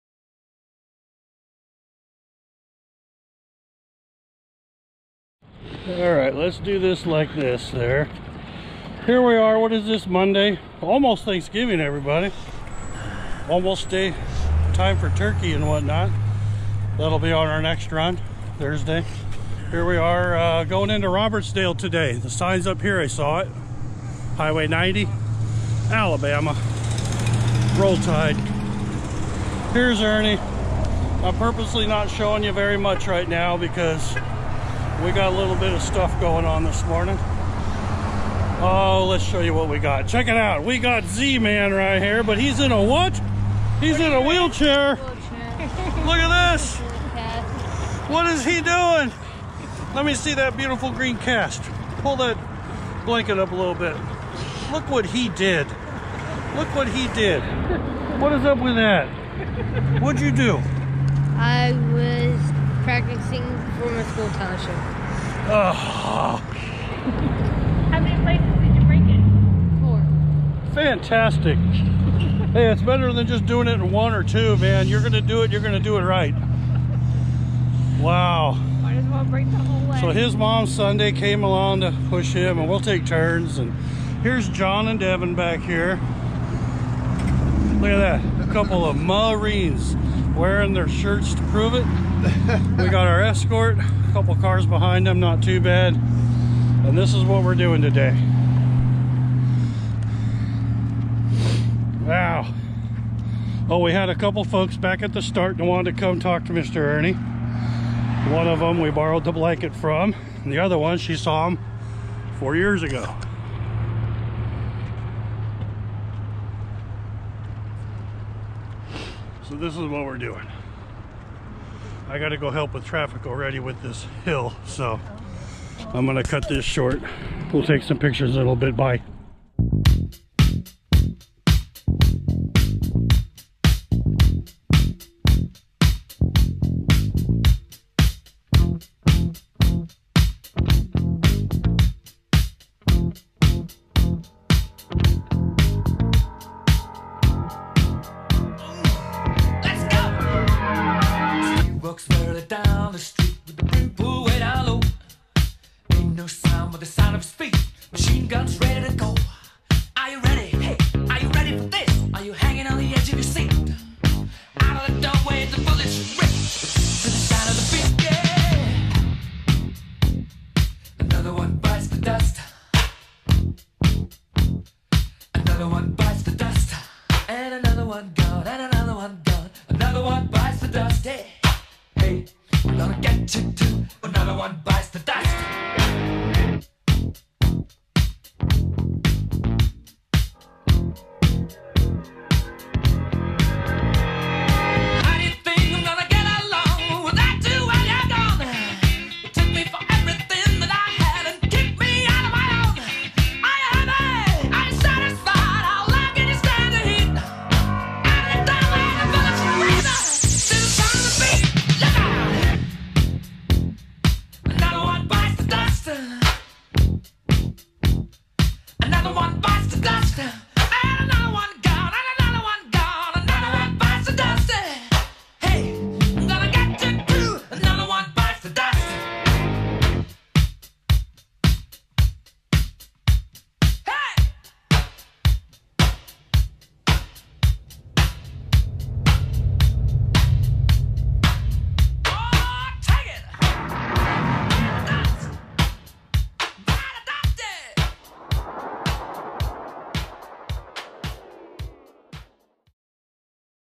More Robertsdale, Alabama!
Ernie and his hearty bunch of runners and supporters bundles up in hoodies and gloves to brave the weather for 4-1/2 miles into and through (part of) Robertsdale, Alabama this morning. One supporter was also having to be carted along in a wheelchair after an unfortunate accident at school but we ALL still had a great time talking and seeing the sights along Highway 90. Next run is on "Turkey Day", so we are not expecting any traffic and then it's on to Thanksgiving dinner in Pensacola, Florida! Happy Holidays Everyone!!!